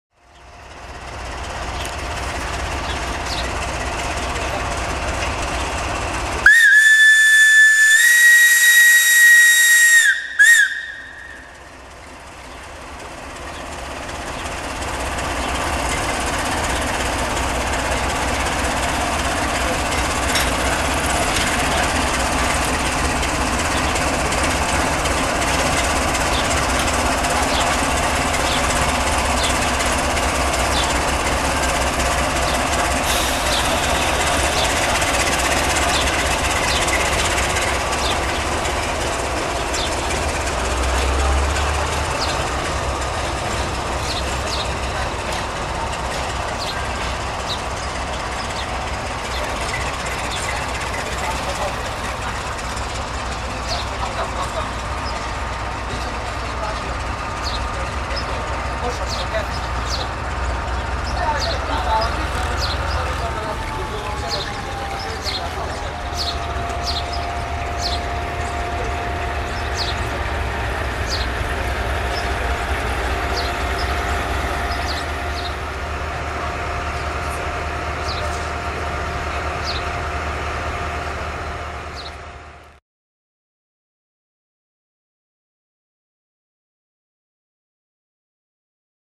Fluieroasa 60-0929-4 in Valea lui Mihai cu Mecanic de Treaba (01 05 2012)
Dedic acest Video tuturor Pasionatilor Feroviari in Romania!

Multumiri mecanicilor pentru FLUIER!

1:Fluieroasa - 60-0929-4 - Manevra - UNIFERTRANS

(c)Train97